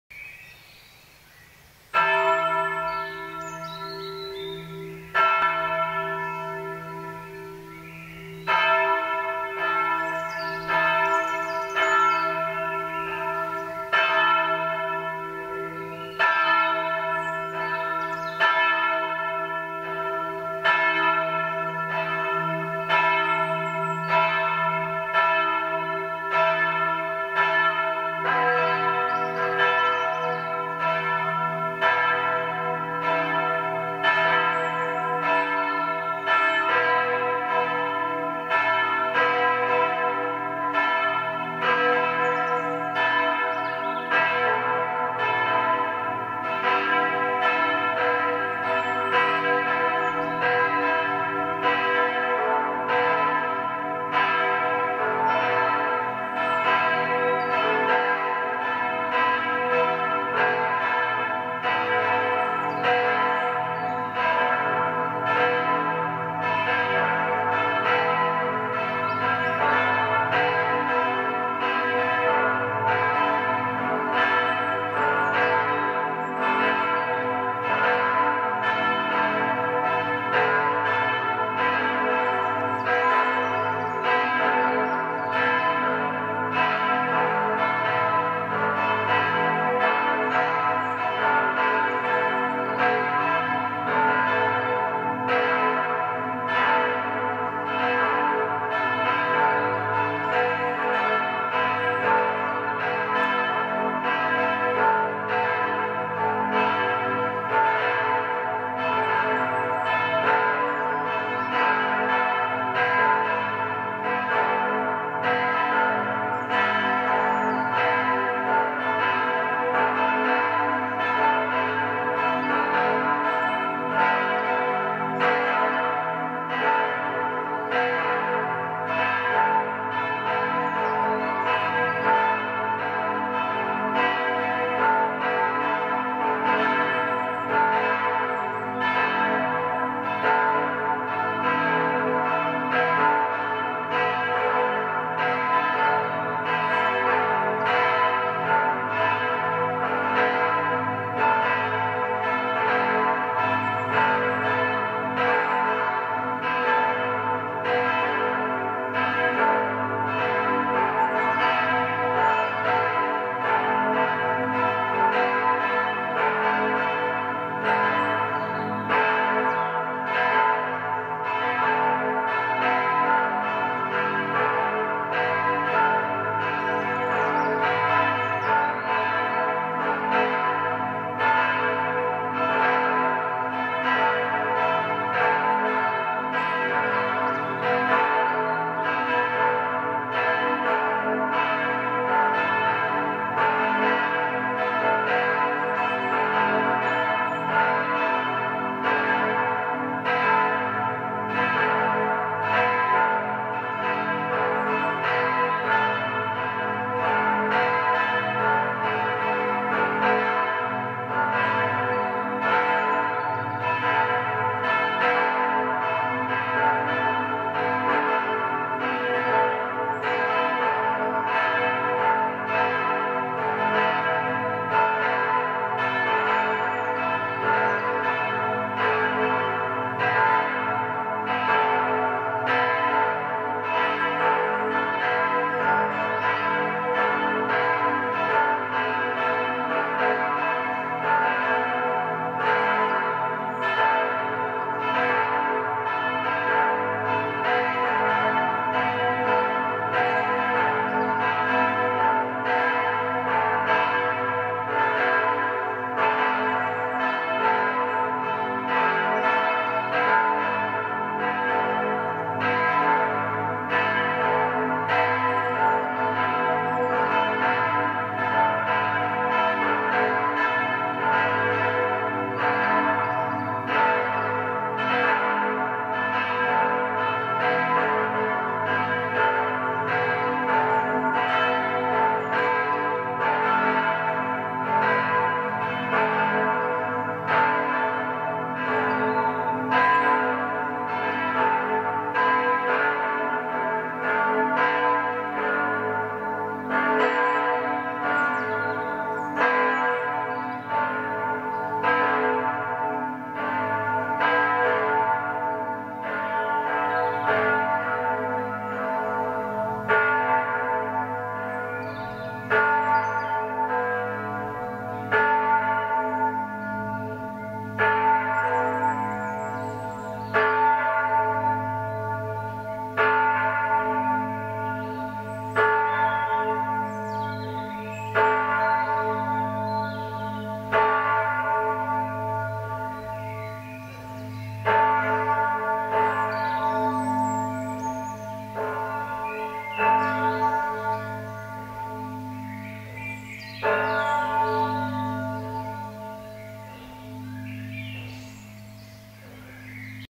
Glocken 2+3+5 der Erzabtei St. Ottilien
(fis°) a° h° (cis) e (fis gis a h)

Läuteanlass:
- Laudes am Palmsonntag

Aufnahme und Foto: Br. Lazarus